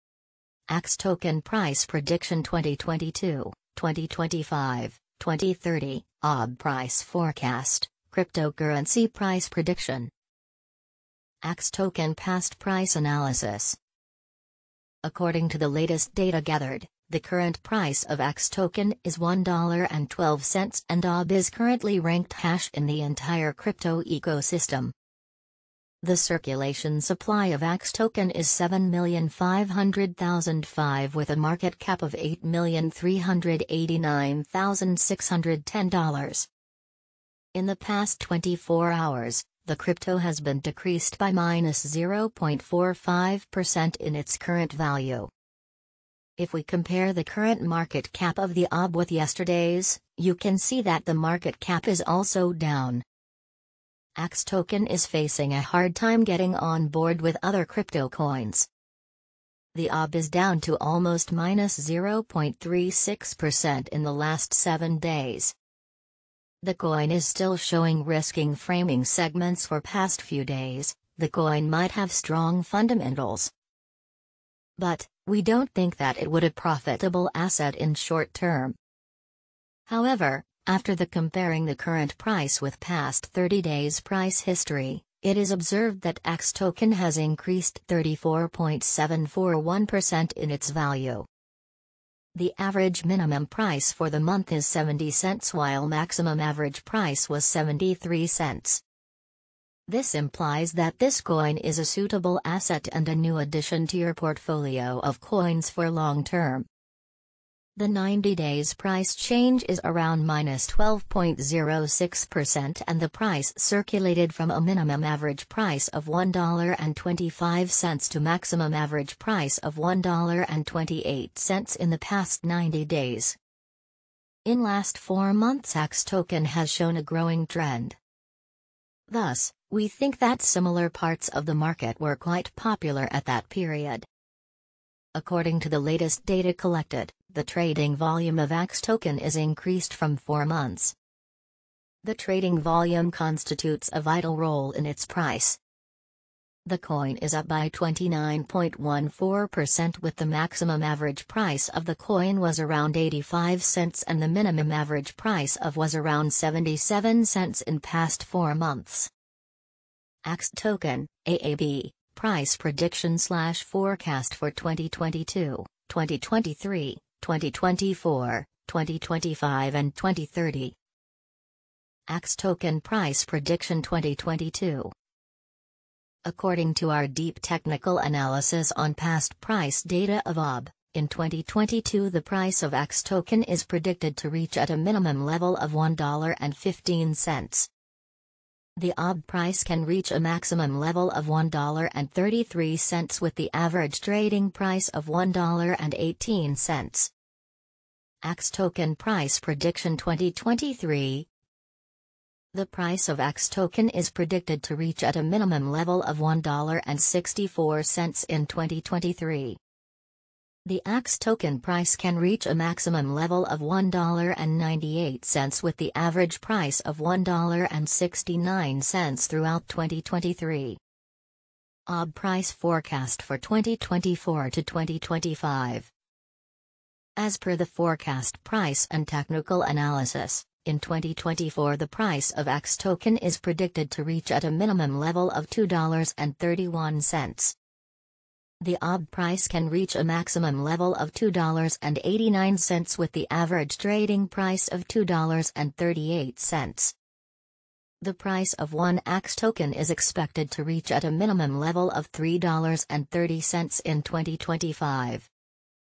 AAX Token Price Prediction 2022, 2025, 2030 AAB Price Forecast Cryptocurrency Price Prediction
AAX Token Past Price Analysis
According to the latest data gathered, the current price of AAX Token is $1.12 and AAB is currently ranked # in the entire crypto ecosystem.
The circulation supply of AAX Token is 7,500,005 with a marketcap of $8,389,610.
 If we compare the current market cap of the AAB with yesterday’s, you can see that the market cap is also down.
AAX Token is facing a hard time getting on board with other crypto coins.
The AAB is down to almost -0.36% in the last 7 days.
However, after the comparing the current price with past 30 days price history, it is observed that AAX Token has increased 34.741% in its value.
The average minimum price for the month is $0.70 while maximum average price was $0.73.
This implies that this coin is a suitable asset and a new addition to your portfolio of coins for long term.
In Last 4 months AAX Token has shown a growing trend.
Thus, we think that similar parts of the market were quite popular at that period.
According to the latest data collected, the trading volume of AAX Token is increased from 4 months.
The trading volume constitutes a vital role in its price.
The coin is up by 29.14% with the maximum average price of the coin was around $0.85 and the minimum average price of was around $0.77 in past 4 months.
AAX Token (AAB) Price Prediction/Forecast for 2022, 2023, 2024, 2025 and 2030
AAX Token Price Prediction 2022
According to our deep technical analysis on past price data of AAB, In 2022 the price of AAX Token is predicted to reach at a minimum level of $1.15.
The AAB price can reach a maximum level of $1.33 with the average trading price of $1.18.
AAX Token Price Prediction 2023
The price of AAX Token is predicted to reach at a minimum level of $1.64 in 2023.
The AAX Token price can reach a maximum level of $1.98 with the average price of $1.69 throughout 2023.
AAB Price Forecast for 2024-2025
As per the forecast price and technical analysis, In 2024 the price of AAX Token is predicted to reach at a minimum level of $2.31.
The AAB price can reach a maximum level of $2.89 with the average trading price of $2.38.
The price of 1 AAX Token is expected to reach at a minimum level of $3.30 in 2025.
The AAB price can reach a maximum level of $4.08 with the average price of $3.42 throughout 2025.
AAX Token Price Prediction 2026
AAX Token price is forecast to reach a lowest possible level of $4.80 in 2026.
As per our findings, the AAB price could reach a maximum possible level of $5.82 with the average forecast price of $4.97.
AAX Token Price Prediction 2027
According to our deep technical analysis on past price data of AAB, In 2027 the price of AAX Token is forecasted to be at around a minimum value of $7.12.
The AAX Token price value can reach a maximum of $8.50 with the average trading value of $7.37 in USD.
AAX Token (AAB) Price Prediction 2028
The price of AAX Token is predicted to reach at a minimum value of $10.30 in 2028.
The AAX Token price could reach a maximum value of $12.50 with the average trading price of $10.60 throughout 2028.
AAX Token Price Prediction/Forecast 2029
As per the forecast and technical analysis, In 2029 the price of AAX Token is expected to reach at a minimum price value of $15.28.
The AAB price can reach a maximum price value of $18.19 with the average value of $15.70.
AAX Token (AAB) Price Prediction 2030
The price of AAX Token is predicted to reach at a minimum value of $23.16 in 2030.
The AAX Token price could reach a maximum value of $26.93 with the average trading price of $23.95 throughout 2030.
AAX Token Price Prediction 2031
AAX Token price is forecast to reach a lowest possible level of $35.21 in 2031.
As per our findings, the AAB price could reach a maximum possible level of $40.73 with the average forecast price of $36.17.

MORE :
Cryptocurrency Price Prediction